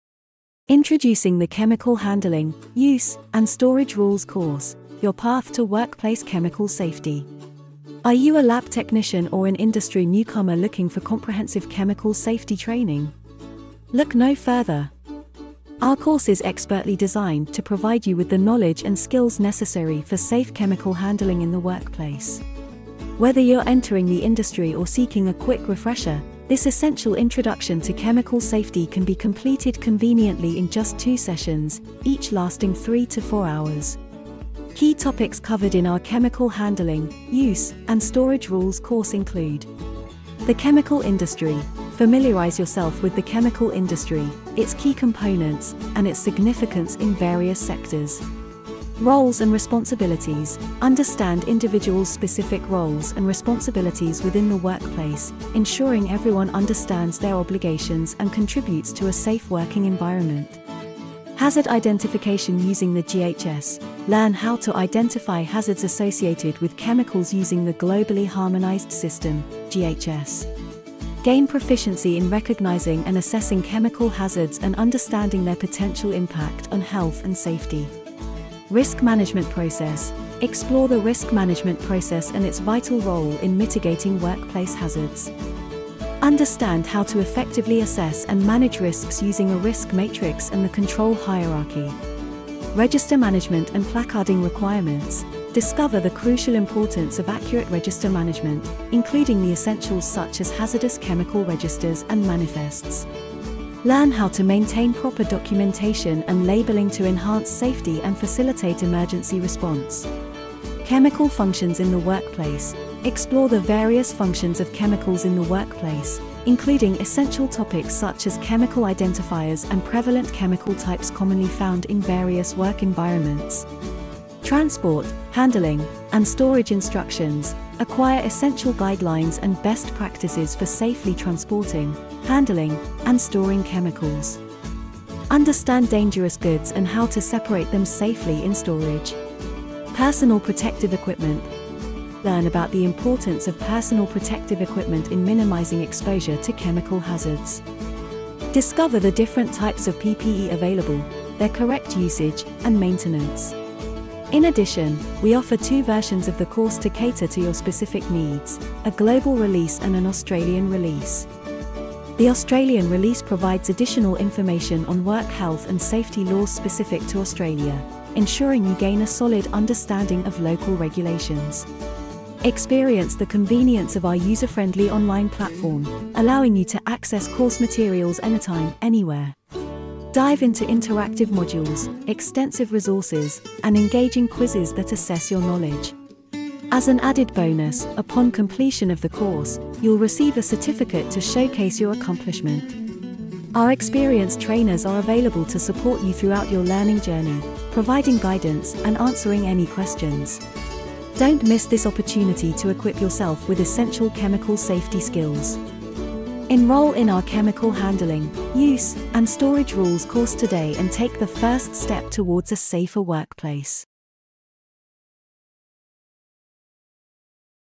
Chemical Handling, Use and Storage Rules | Online Course for Lab Technicians
Welcome to our Chemical Handling, Use, and Storage Rules course! This short, comprehensive online course is designed to provide you with essential knowledge of chemical safety and best practices for handling, using, and storing hazardous chemicals.

Course Highlights:

    🖥️ Fully online and self-paced
    ⏳ Two short sessions (2-3 hours each)
    📝 Interactive eLearning modules and self-assessment quizzes
    📚 Immediate access to all course content and reading materials
    🕒 Access anytime, with your progress saved when you leave
    📅 Three months to complete the course
    📖 Learner guides and additional industry resources included
    ✅ Multiple-choice quiz to measure your understanding
    🎓 Certificate of completion awarded

Course Topics:

    📜 Work Health and Safety Laws
    🌐 Overview of the Chemical Industry
    👥 Roles and Responsibilities in the Workplace
    ⚠️ Hazard Identification Using GHS
    🔍 Risk Management Process and Exposure Risks
    🗂️ Management of Registers and Placarding Requirements
    🧪 Chemical Functions in the Workplace
    🚚 Transport, Handling, and Storage Instructions
    🦺 Personal Protective Equipment (PPE)

Perfect for lab technicians, graduates, material scientists, and anyone new to the chemical industry, this course is your go-to introduction to workplace chemical safety.